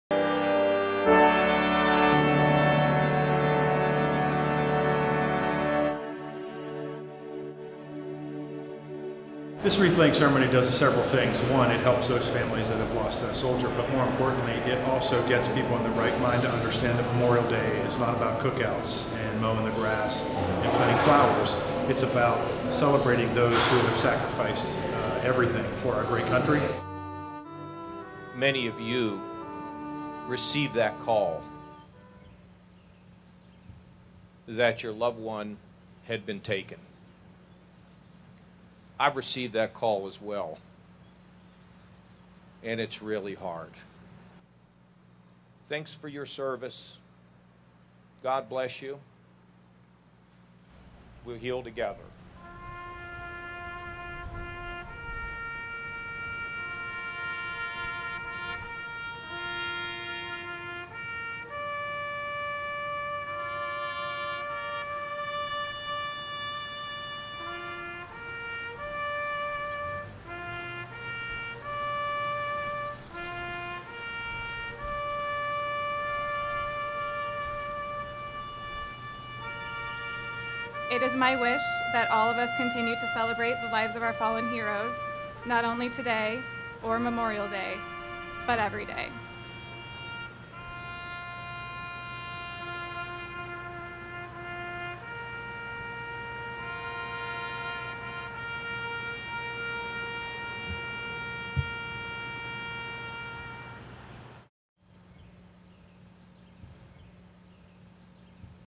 ODVS Wreath Laying Ceremony
On May 21, 2018, the State of Ohio honored the courageous Ohio service members who lost their lives while allowing us to continue to live our lives in freedom. Ohio Governor John Kasich and Department of Veterans Services Director Chip Tansill as well as Gold Star Wife Brittnay McCall offered moving tributes to these men and women during the formal ceremony.